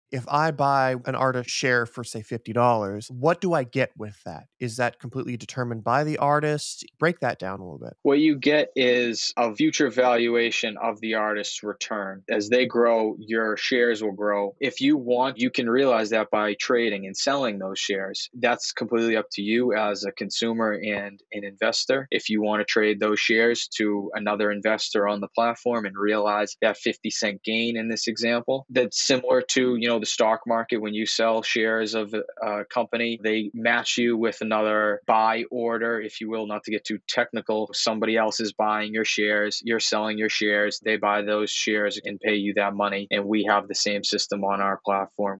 What Do Fans Get After Investing in an Artist?
Justin Longo, CEO of MariNation Music, explains what fans receive after they've purchased shares in an artist.

27 Creative Ways to Market Your Music and Get Heard (PDF Guide)